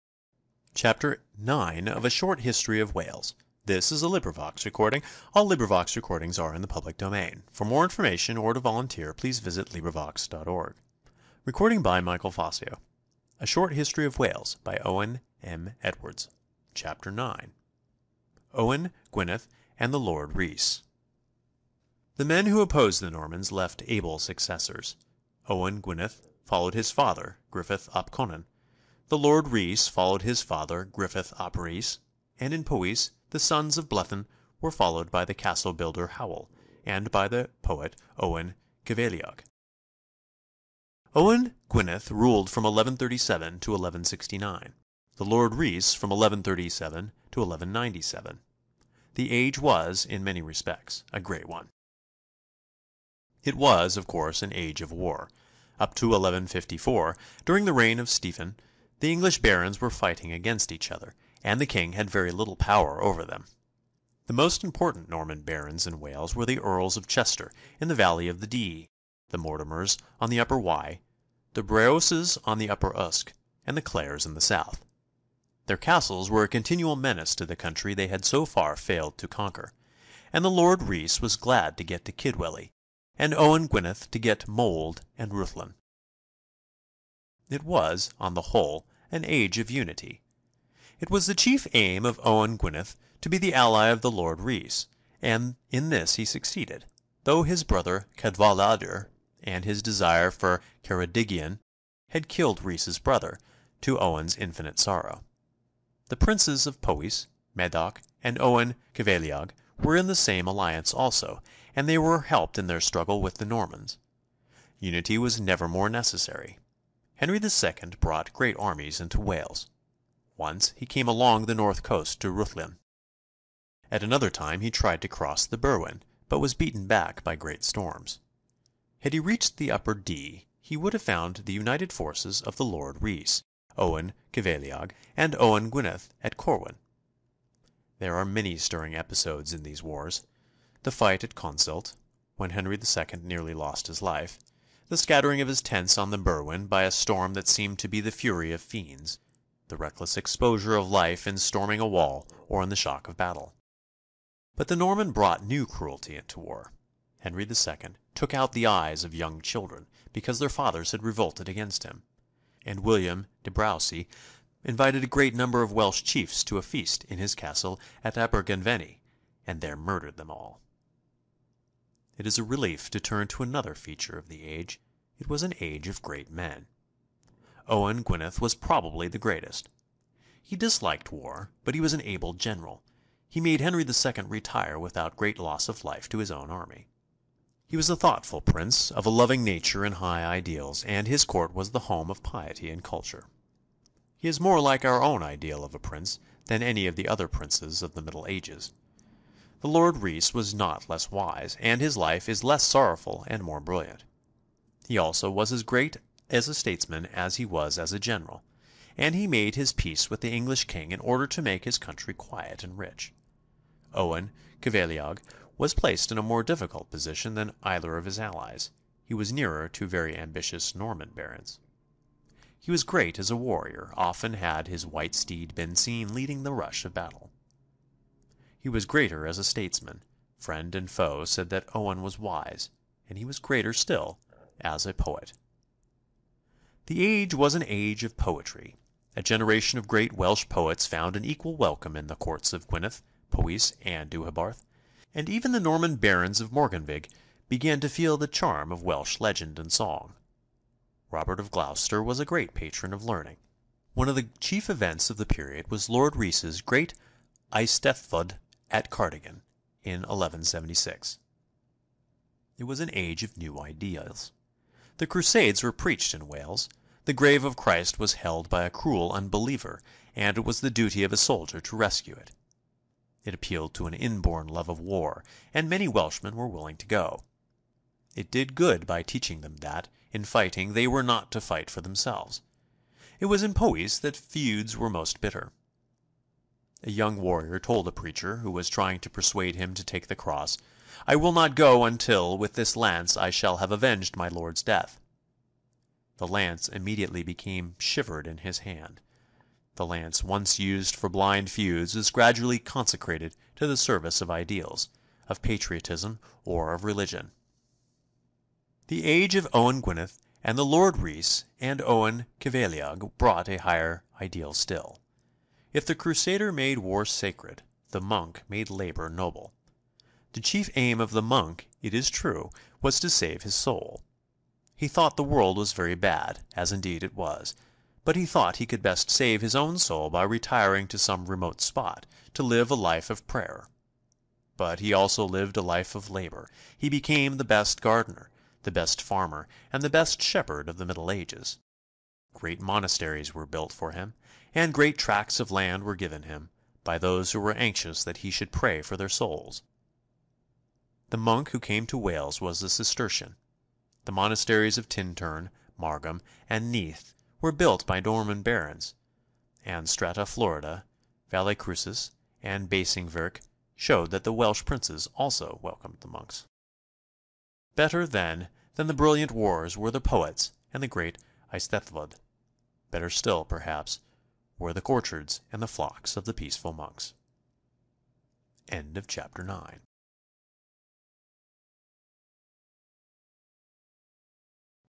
Owen Edwards - A Short History Of Wales (9/25) Owen Gwynedd And The Lord Rees
A SHORT HISTORY OF WALES.
Is a book published in 1906 by Owen Edwards. It aimed to provide an introduction to Welsh history, to those who may not have previously dealt with the subject.

AUTHOR BIOGRAPHY.
Sir Owen Morgan Edwards (December 26, 1858 – May 15, 1920) was a Welsh historian, educationalist and writer. He is often known as O. M. Edwards. Owen Edwards was born in Llanuwchllyn near Bala, the eldest son of Owen and Beti Edwards. He gives a vivid description of his early education in his autobiography Clych Adgof ("Bells of Remembrance") published in 1906. Llanuwchllyn was then and remains a strongly Welsh-speaking area, but the Welsh Not was in use at the local school and usually found itself round Owen's neck, as a punishment for speaking Welsh.

After studying at Bala and University College of Wales, Aberystwyth, graduating with a pass degree in 1883. He then spent a year at the University of Glasgow studying philosophy before studying at Balliol College, Oxford from 1884 to 1887, winning all the university's three main prizes for history. He came under the influence of the Dafydd ap Gwilym Society at Oxford, establishing friendships with other students who later became prominent in Welsh life such as John Morris-Jones and becoming a strong cultural nationalist. He then spent a year on the continent before returning to Oxford, where he was appointed a Fellow of Lincoln College and became tutor in history there and at other colleges. He remained in this position until 1907.

In 1907 he was appointed as the first Chief Inspector of Schools for Wales under the recently established Welsh Education Department. He took the first steps towards ensuring that the Welsh language was taught in the schools of Wales. He came into conflict with the Central Welsh Board because of his conviction that the new intermediate schools were anglicising influences in Wales and his determination to resist this. He was also a prolific writer, starting and editing the periodical Cymru in 1891 and Cymru'r Plant for children in 1892, intended to arouse interest among the people of Wales in their own language and history. An English language version of Cymru, Wales (1901), was less successful, although it is seen as "his major work". He wrote a number of books on Welsh history, notably Cartrefi Cymru ("Welsh Homes") which describes visits to the homes of a number of important historical figures, and published the works of other writers in a series of Welsh classics Cyfres y Fil ("The Series of a Thousand" - in thirty seven volumes, so called because the author sought one thousand subscribers, through the periodical Cymru).

Edwards was elected as a Liberal Member of Parliament for Merionethshire following the premature death of T. E. Ellis MP in April 1899. He did not enjoy parliamentary life and did not seek re-election in 1900. He was knighted in 1916 and awarded the degree of D.Litt. by the University of Wales in 1918. Edwards died at Llanuwchllyn in 1920, his wife Ellen having died the previous year. His first son died at the age of five. His second son, Ifan ab Owen Edwards, founded Urdd Gobaith Cymru ("The Welsh League of Youth"). His only daughter Hâf married Sir David Hughes Parry.